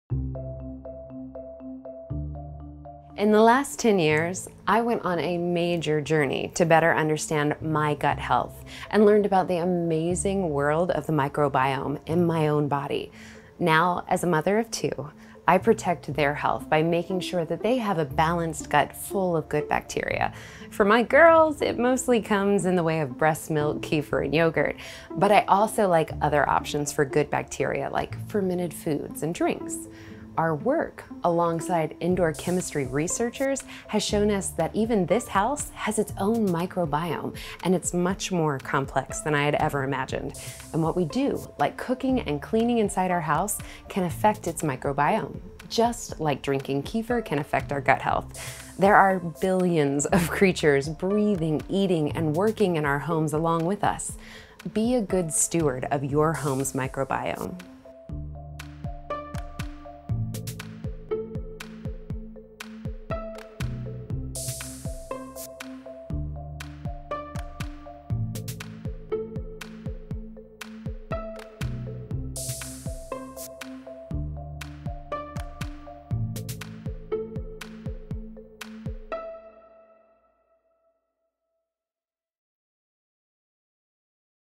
Bugs in Your Home: Indoor Microbiology 101
The HOMEChem Experiment, along with many others, have shown that indoor environments are as much of a jungle as an actual jungle- there's an ecosystem that's very complex at work all around us, in every room of every home (and school, and office, etc, etc). We don't have to be afraid of this ecosystem, but we should be aware of it if we want to navigate a healthy way to coexist without all the nasty spikes in mold, mildew, dust mites, and more.
Get your home analyzed for free, and explore what you can do about your indoor environment